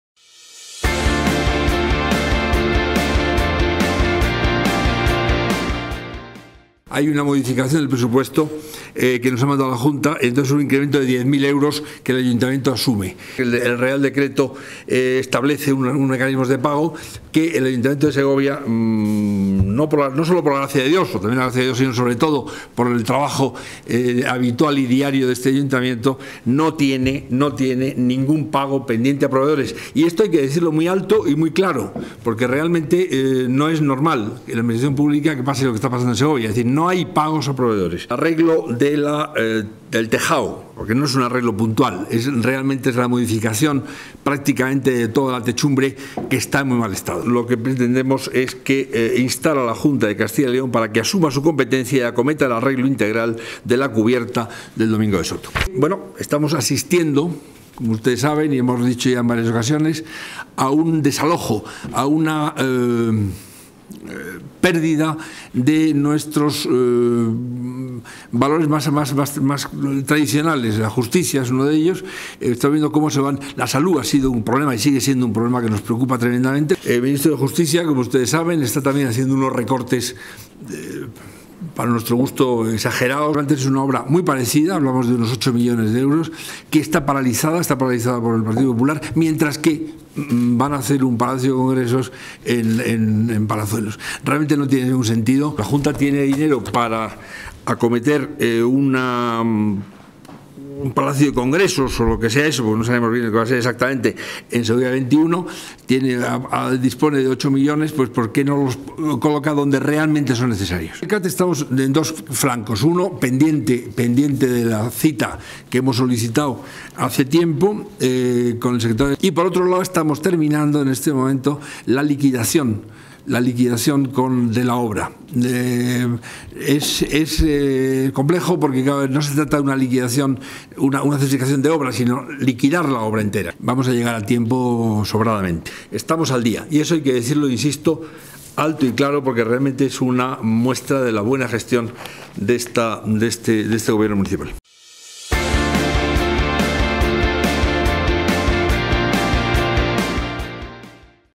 El PSOE instará a la Junta para que acometa los asuntos de Segovia que son de su competencia
El Grupo Socialista del Ayuntamiento de Segovia de la mano de su Portavoz, Javier Giráldez ha presentado hoy 4 mociones que presentarán de cara al pleno que se celebrará este miércoles a las 9:00 h. de la mañana.

Cámara y edición: Azahara Raigada